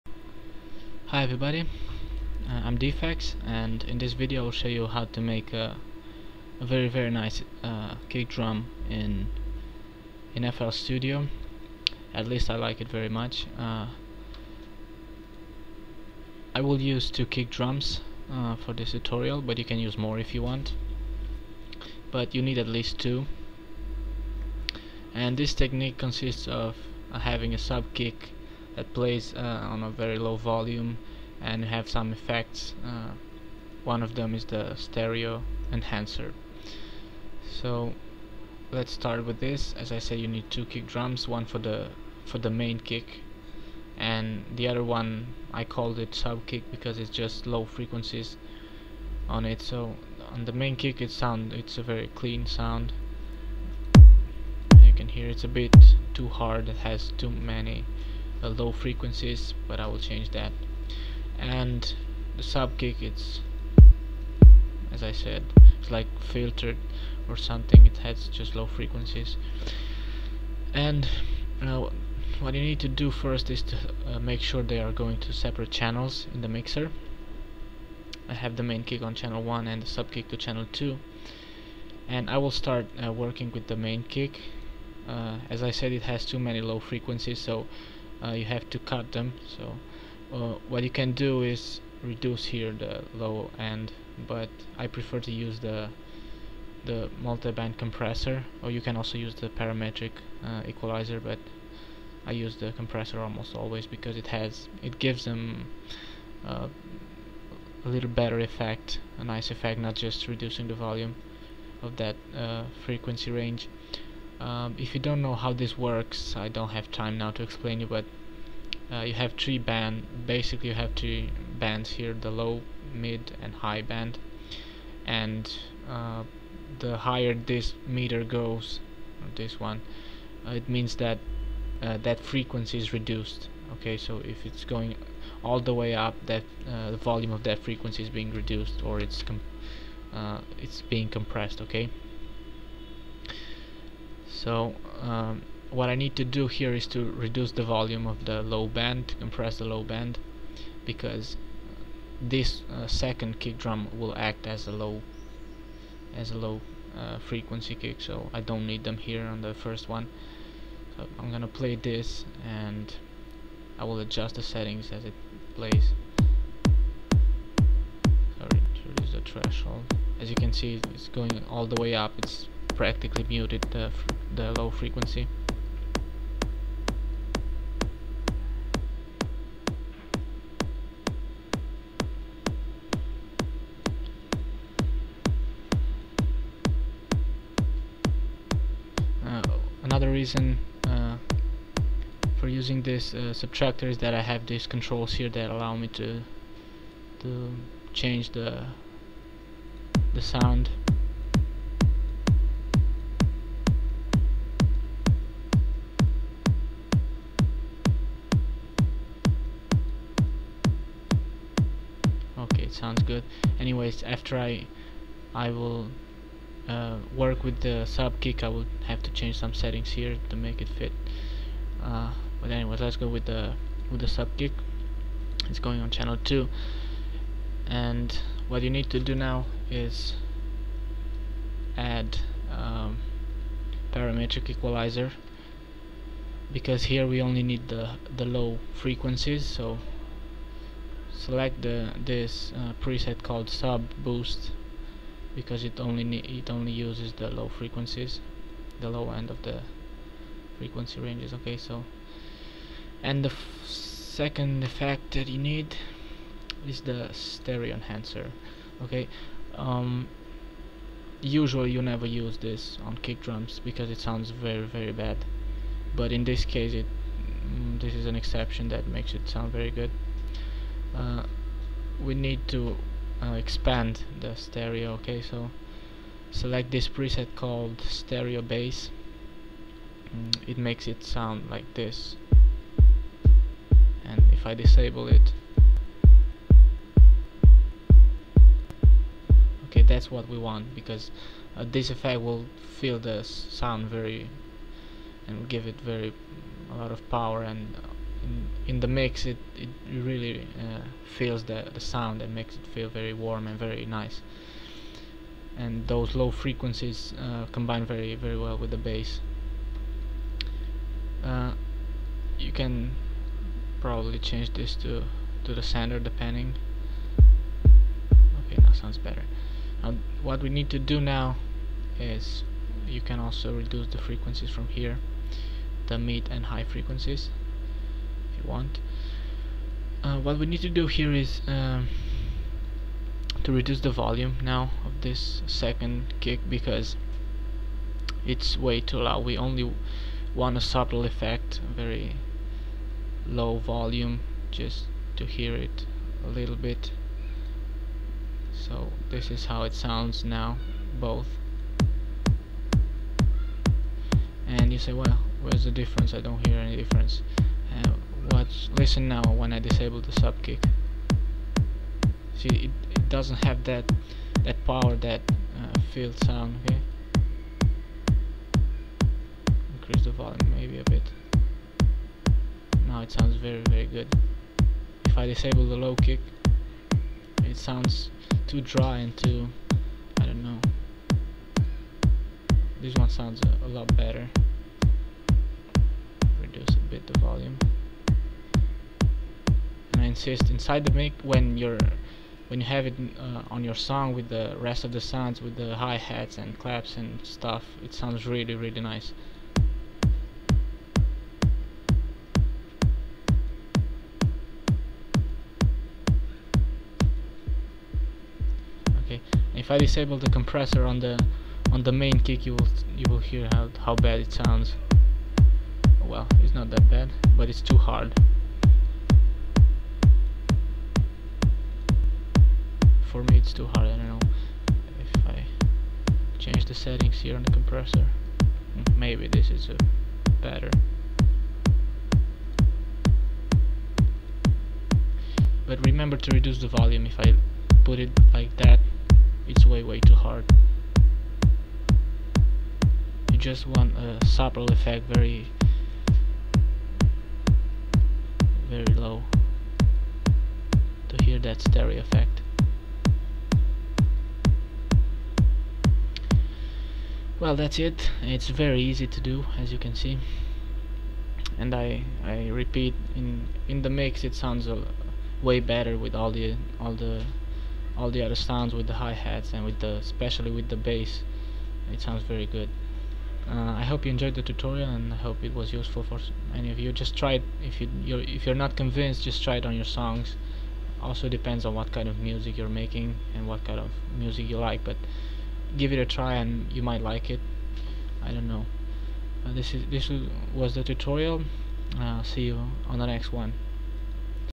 Awesome kick drums sound technique in FL studio (watch fullscreen)
Give depth and power to your mixes with this sound effect on the kick drums